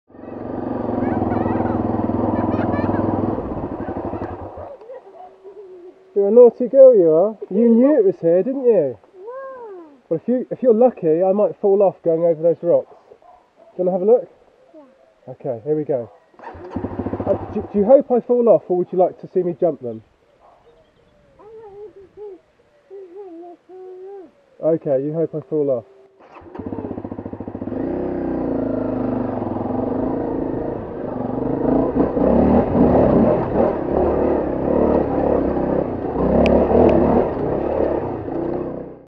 Biker vs Militant Walker
This laughing walker lied about the byway location, obstructed the highway and hoped that I fell off! 😡

Dartmoor, 21.04.19